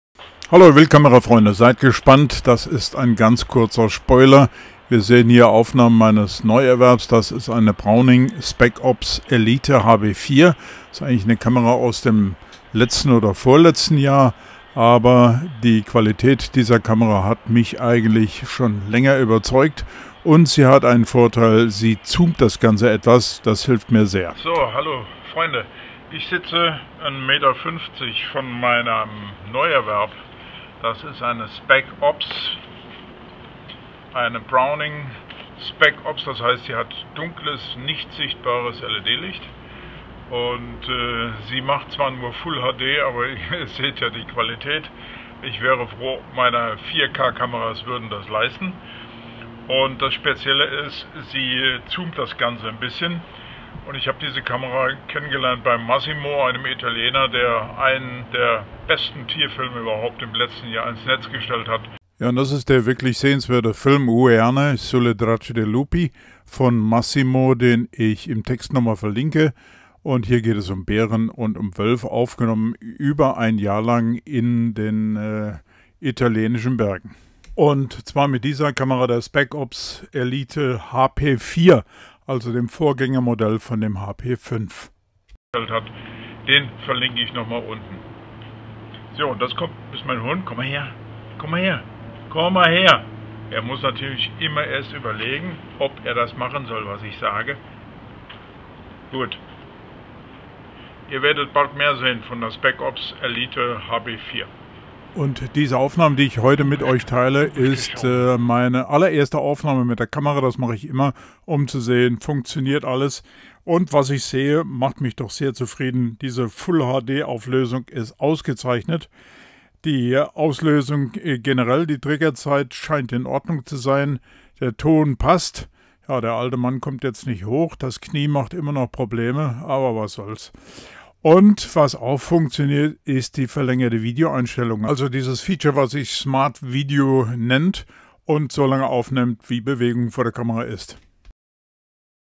Gerade eingetroffen Browning Spec OPS Elite HP4 allererster Test. Bald mehr
Darauf habe ich mich gefreut und ich glaube zu Recht. Die ersten Aufnahmen meines Neuerwerbs, der Browning Spec Ops Elite HP4 sind viel versprechend. Ich hoffe die Nachtaufnahmen enttäuschen mich nicht.
Bild und Ton sind sehr gut und der Aufnahmewinkel ist das was ich gut brauchen kann. Das Wild kommt etwas näher zur Kamera - will heißen - die Kamera kann etwas weg sein.
Die Kamera wird auch im Film "UN ANNO SULLE TRACCE DEI LUPI" also, ein Jahr auf den Spuren der Wölfen, verwendet. Aber Massimo hat nicht nur tolle Wolfsaufnahmen in diesem Film, sondern auch Braunbären. Ein sehenswerter Film mit den richtig wilden Tieren Europas:
Dank geht an @Beholdingnature  mit dem Wunsch auf einen neuen Film.
UN SECONDO ANNO SULLE TRACCE DEI LUPI.